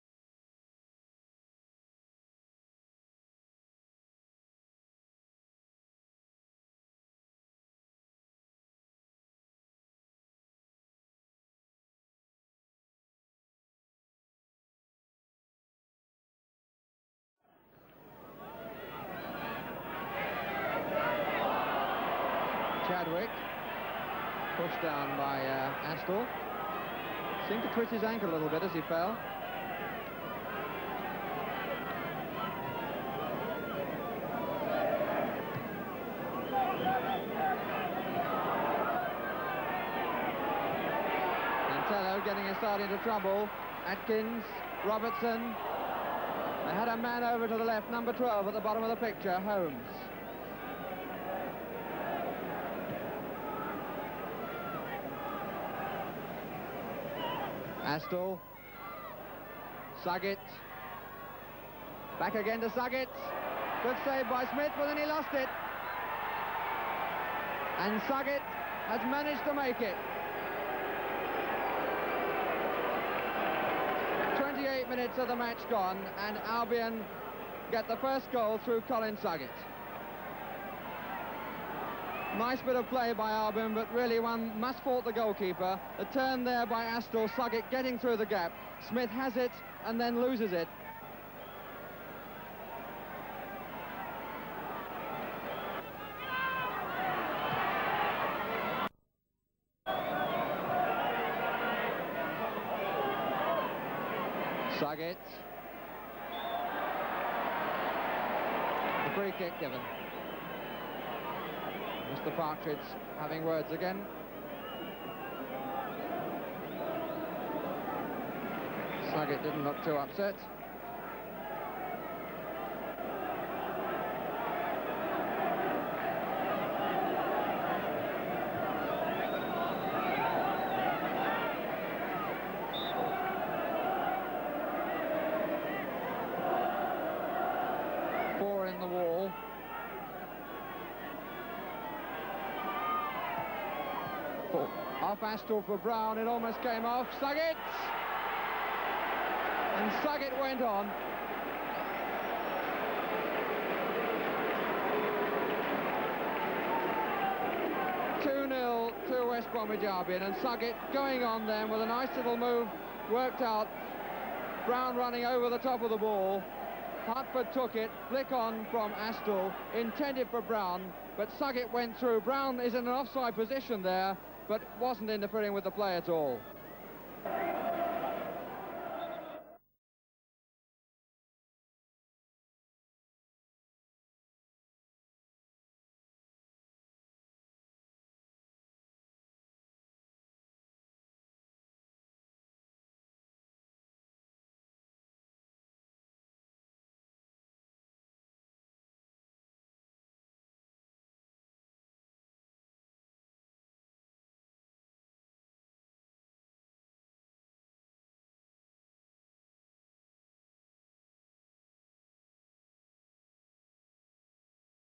Halifax Town 0 v 2 West Bromwich Albion (Watney Cup 1971)
New manager Don Howe sent his West Brom team out determined there was to be no repeat of Halifax Town's Round 1 giant-killing of Manchester United. A "robust" performance, that became the hallmark of Don Howe teams, saw them win a place in the final, where they were beaten by the Colchester United team that had achieved FA Cup immortality by sensationally defeating Don Revie's mighty Leeds United.